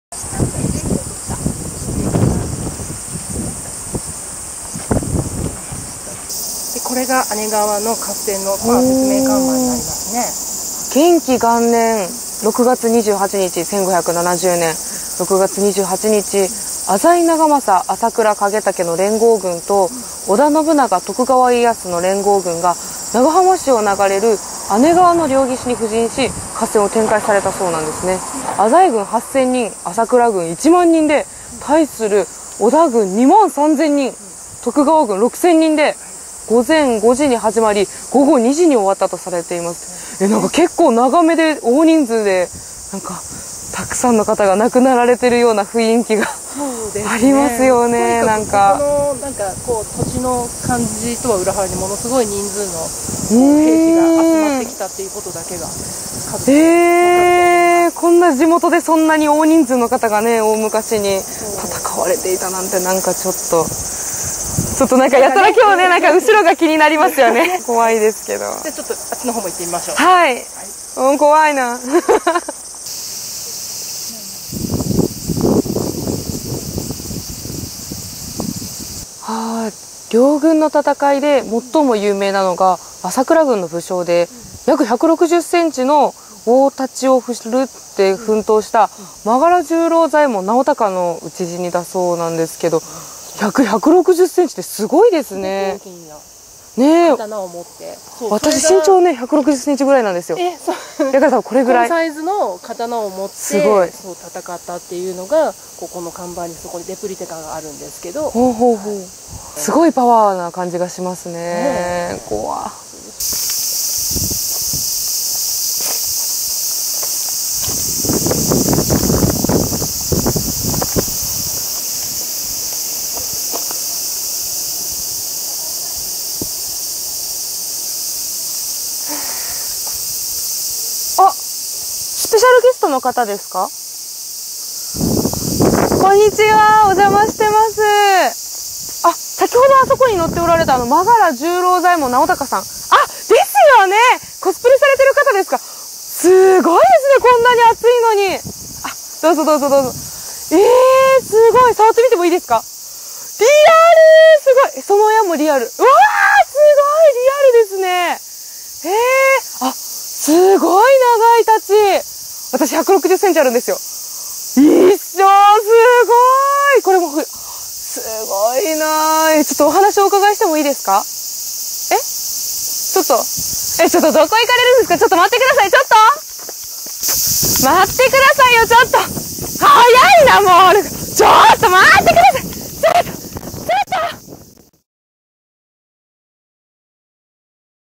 湖北の怖い話～「血原」姉川古戦場跡
寝苦しい真夏の夜にひんやりする、湖北の怖い話。今回は、長浜市の「血原」。「姉川の合戦」跡地は景色のきれいな公園。レポーターは「もえりーぬ」。が、他に誰もいないのに突然、彼女が戦国武将と話しはじめた！？さて・・・
2016年8月収録